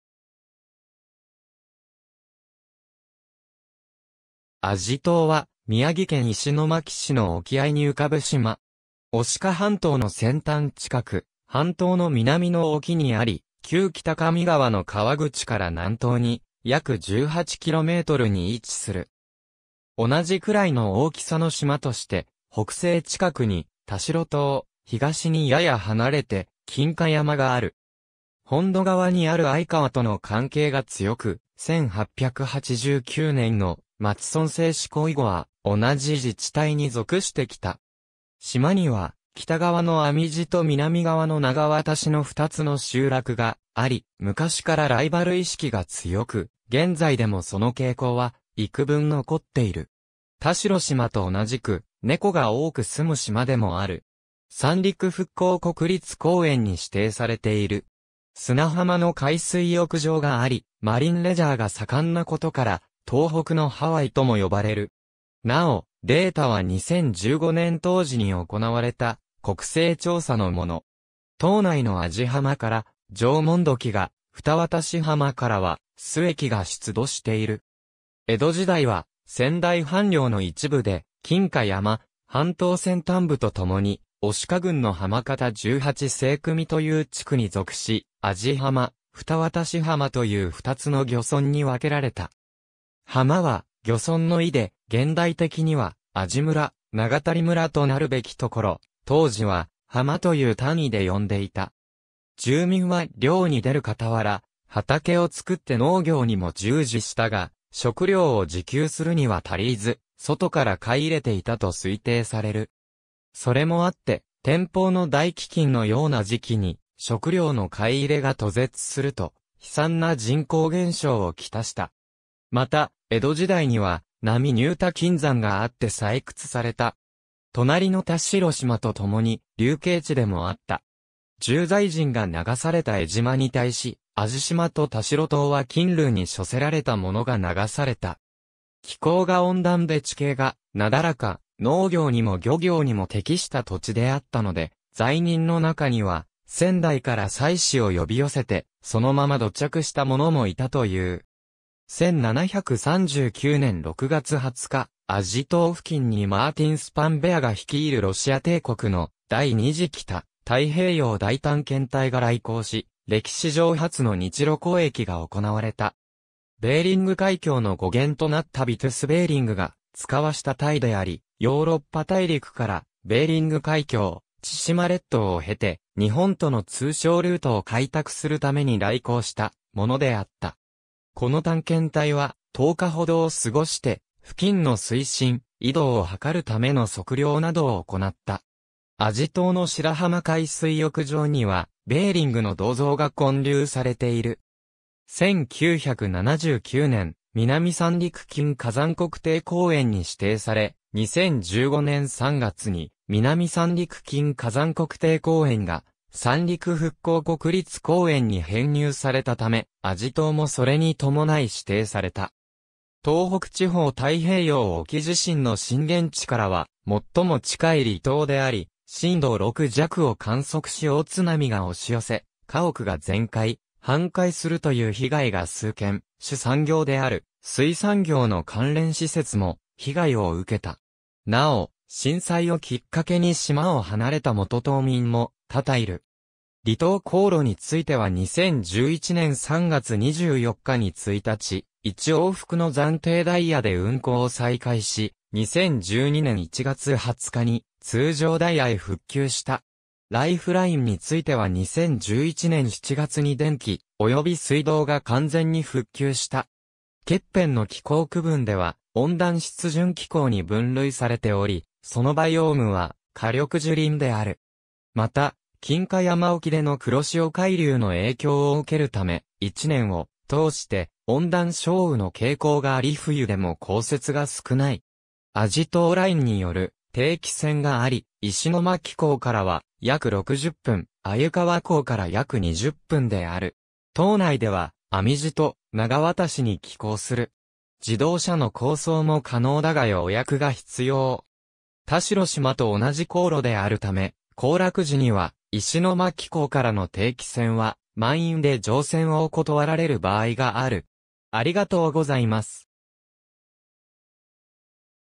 網地島
網地島（あじしま）は、宮城県石巻市の沖合いに浮かぶ島。
牡鹿半島の先端近く、半島の南の沖にあり、旧北上川の河口から南東に約18kmに位置する。
同じくらいの大きさの島として、北西近くに田代島、東にやや離れて金華山がある。
本土側にある鮎川との関係が強く、1889年の町村制施行以後は同じ自治体（牡鹿町および石巻市）に属してきた。
島には、北側の網地と南側の長渡（ふたわたし）の2つの集落があり、昔からライバル意識が強く、現在でもその傾向は幾分残っている。
田代島と同じく、猫が多く住む島でもある。
三陸復興国立公園に指定されている。
砂浜の海水浴場がありマリンレジャーが盛んなことから「東北のハワイ」とも呼ばれる。
なお、データは2015年当時に行われた国勢調査のもの。
島内の網地浜から縄文土器が、長渡浜からは須恵器が出土している。
江戸時代は仙台藩領の一部で、金華山、半島先端部とともに牡鹿郡の浜方十八成組という地区に属し、網地浜、長渡浜という二つの漁村に分けられた。
浜は漁村の意で、現代的には網地村・長渡村となるべきところ、当時は浜という単位で呼んでいた。
住民は漁に出るかたわら、畑を作って農業にも従事したが、食糧を自給するには足りず、外から買い入れていたと推定される。
それもあって、天保の大飢饉のような時期に食糧の買い入れが途絶すると、悲惨な人口減少をきたした。
また江戸時代には浪入田金山があって採掘された。
隣の田代島とともに流刑地でもあった。
重罪人が流された江島に対し、網地島と田代島は近流に処せられたものが流された。
気候が温暖で地形がなだらか、農業にも漁業にも適した土地であったので、罪人の中には、仙台から妻子を呼び寄せて、そのまま土着した者もいたという。
1739年（元文4年）6月20日、網地島付近にマーティン・スパンベアが率いるロシア帝国の第2次北太平洋大探検隊が来航し、歴史上初の日露交易が行われた（元文の黒船）。
ベーリング海峡の語源となったヴィトゥス・ベーリングが遣わした隊であり、ヨーロッパ大陸からベーリング海峡、千島列島を経て日本との通商ルートを開拓するために来航したものであった。
この探検隊は、10日ほどを過ごして付近の水深・緯度を測るための測量などを行った。
網地島の白浜海水浴場には、ベーリングの銅像が建立されている。
1979年（昭和54年）、南三陸金華山国定公園に指定され、2015年（平成27年）3月に南三陸金華山国定公園が三陸復興国立公園に編入されたため網地島もそれに伴い指定された。
・東北地方太平洋沖地震の震源地からは最も近い離島であり、震度6弱を観測し大津波が押し寄せ、家屋が全壊・半壊するという被害が数件、主産業である水産業の関連施設も被害を受けた。
なお、震災をきっかけに島を離れた元島民も多々いる。
・離島航路については2011年3月24日に1日1往復の暫定ダイヤで運航を再開し、2012年1月20日に通常ダイヤへ復旧した。
・ライフラインについては2011年7月に電気及び水道が完全に復旧した。
ケッペンの気候区分では温暖湿潤気候（Cfa）に分類されており、そのバイオームは夏緑樹林である。
また、金華山沖での黒潮海流の影響を受けるため、一年をとおして温暖少雨の傾向があり冬でも降雪が少ない。
網地島ラインによる定期船があり、石巻港からは約60分、鮎川港から約20分である。
島内では網地と長渡に寄港する。
自動車の航送も可能だが予約が必要。
田代島と同じ航路であるため、行楽時には石巻港からの定期船は満員で乗船を断られる場合がある。